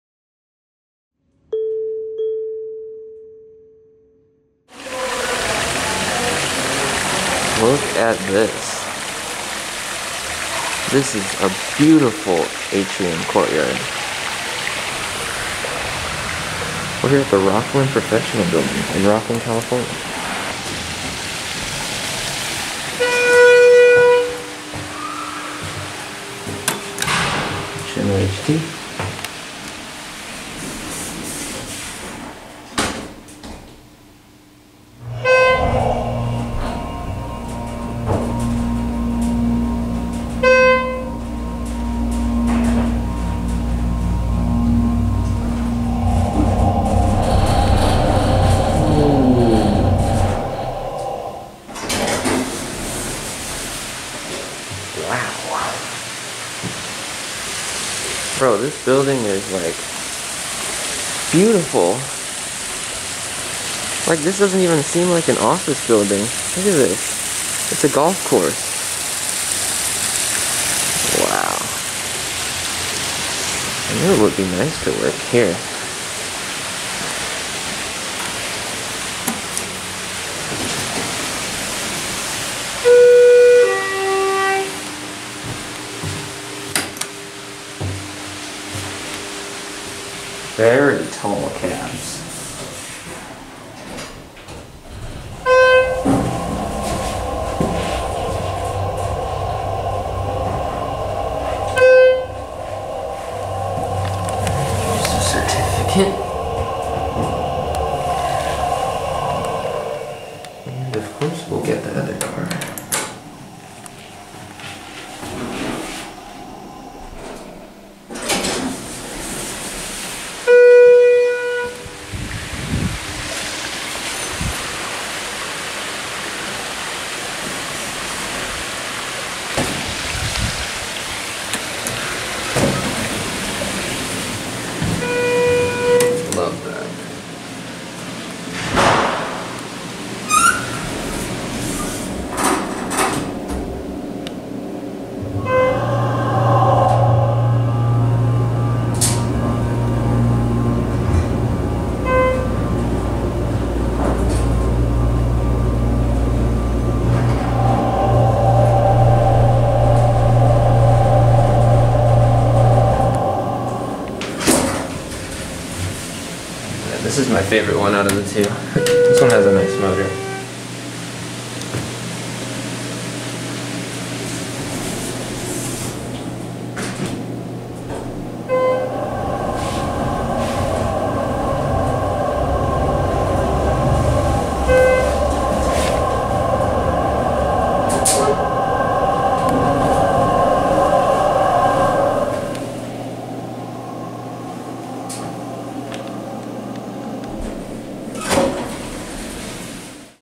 Nice Schindler Hydraulic Elevators - Rocklin Professional Building - Rocklin, CA
[Recorded 4-9-21] This is a really neat building! You wouldn’t expect anything like this to be inside from looking at it from the parking lot. Upon entering the main entrance, you’re greeted with a nice water feature that spans the entire atrium…and a mini golf course?!? Yup! This random office building has its own full mini golf course inside of it. Must be fun working here lol. The elevators are basic but nice Schindler 330A hydros with HT fixtures